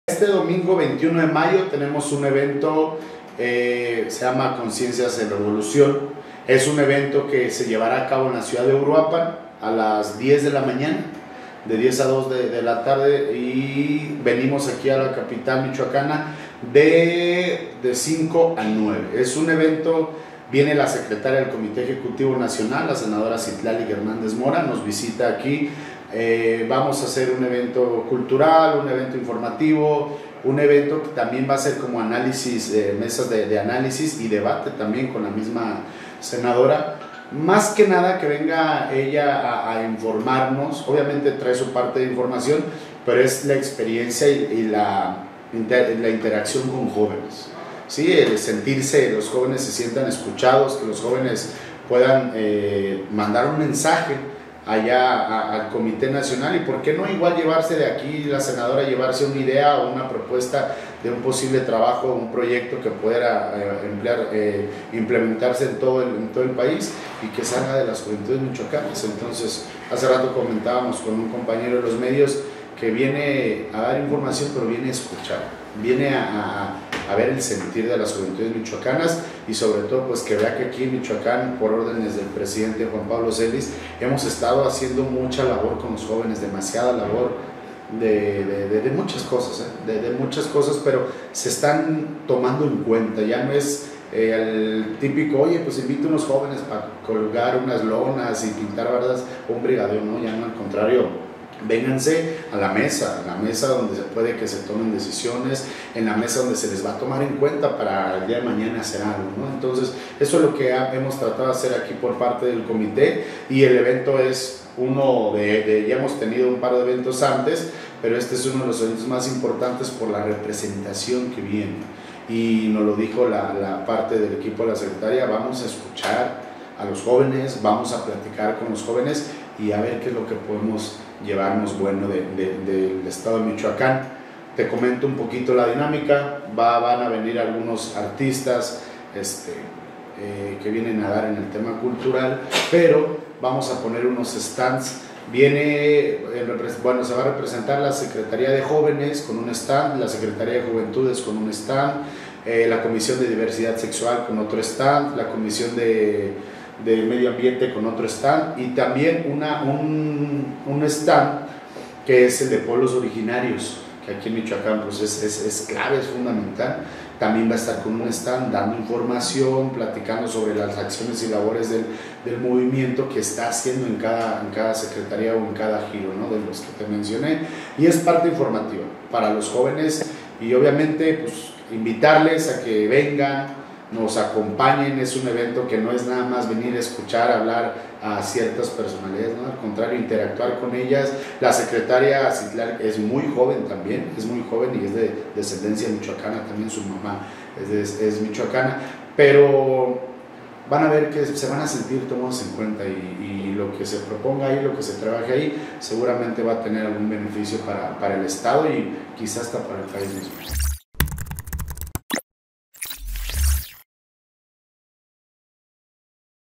Invita Morena Michoacán a Conciencias en Revolución
La secretaria general de Morena a nivel nacional, Citlalli Hernández Mora, visitará este domingo Michoacán para escuchar e interactuar con las juventudes del partido en el evento denominado «Conciencias en Revolución», dirigido a jóvenes de entre 18 y 35 años de edad.